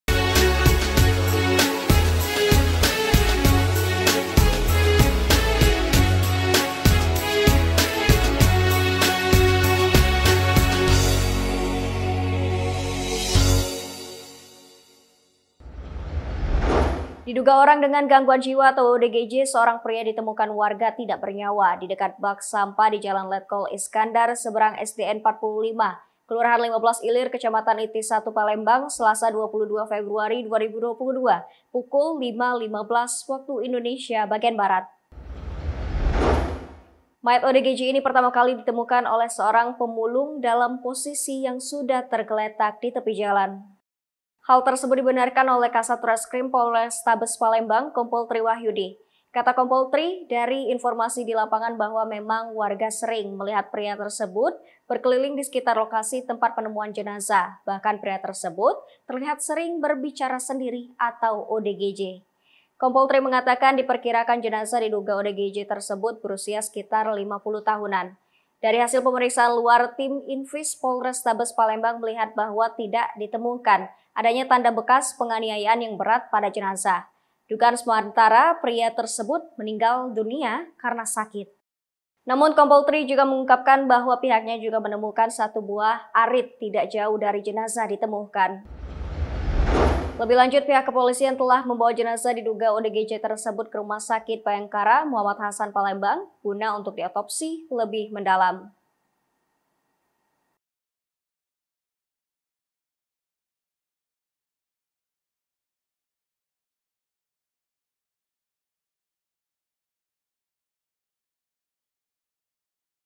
Mati Nahas, Mayat ODGJ Tergeletak Dipinggir Jalan
Diduga Orang Dengan Gangguan Jiwa (ODGJ), seorang pria ditemukan warga tidak bernyawa di dekat bak sampah di Jalan Letkol Iskandar Seberang SDN 45 Kelurahan 15 Ilir Kecamatan IT 1 Palembang, Selasa 22 Februari 2022 pukul 05.15 WIB.

Mayat OGDJ ini pertama kali ditemukan oleh seorang pemulung dalam posisi yang sudah tergeletak di tepi jalan.

Hal tersebut dibenarkan Kasat Reskrim Polrestabes Palembang, Kompol Tri Wahyudi. Kata Kompol Tri, dari informasi di lapangan bahwa memang warga sering melihat pria tersebut berkeliling di sekitar lokasi tempat penemuan jenazah, bahkan pira tersebut terlihat sering berbicara sendiri atau ODGJ.

Kompol Tri mengatakan, diperkirakan jenazah diduga ODGJ tersebut berusia sekitar 50 tahun. Dari hasil pemeriksaan luar tim infis Polrestabes Palembang melihat bahwa tidak ditemukan adanya tanda bekas penganiayaan yang berat pada jenazah. Dugaan sementara pria tersebut meninggal karena sakit.

Namun, Kompol Tri juga mengungkapkan bahwa pihaknya juga menemukan satu buah arit tidak jauh dari jenazah ditemukan.

Lebih lanjut, pihak kepolisian telah membawa jenazah diduga ODGJ tersebut ke rumah sakit Bhayangkara Moh Hasan Palembang gun autopsi lebih mendalam.
Akun official resmi Infoindonesia.id